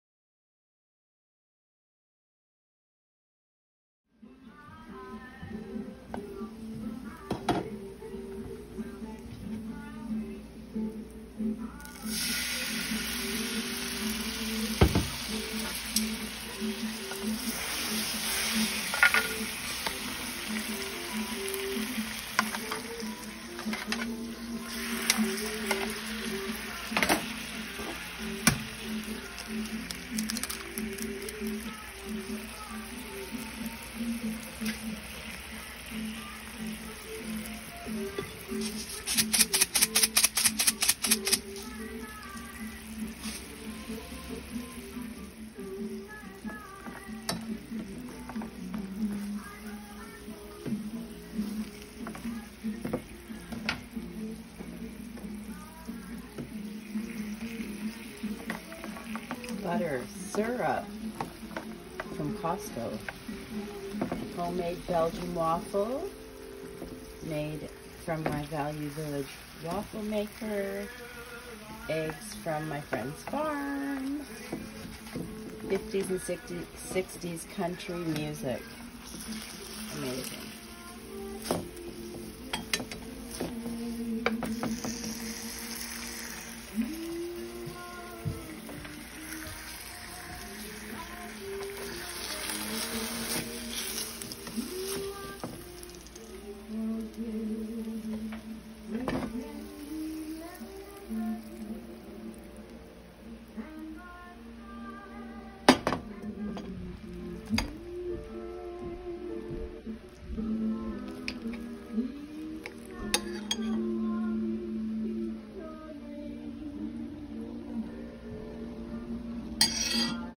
Homemade Belgium Waffles Dressed Up
With unsalted cashews maple 🍁 syrup and farm fresh egg 🥚 from my friend’s organic